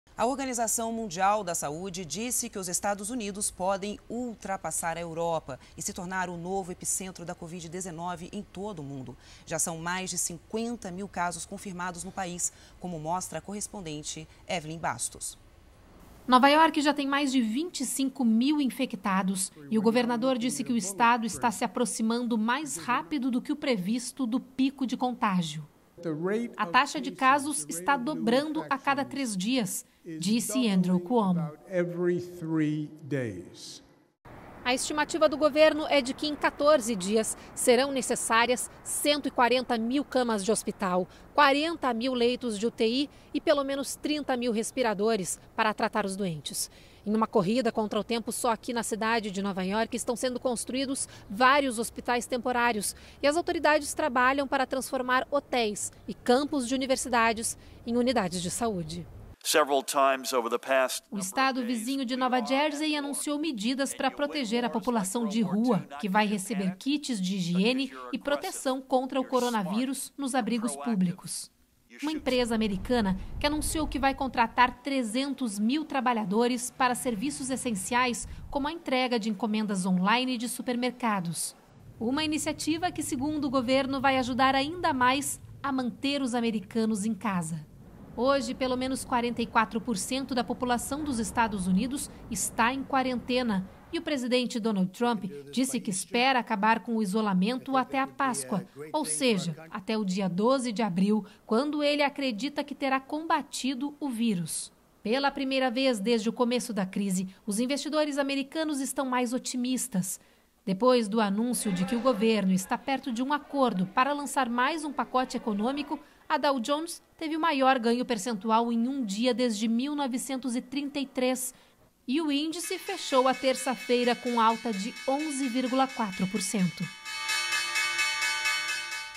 Estados Unidos podem se tornar novo epicentro do coronavírus
Nova York já tem mais de 25 mil infectados e a taxa de casos está dobrando a cada três dias. Além da construção de hospitais temporários, o governo de Nova York trabalha para transformar hotéris e universidades em unidades de saúde.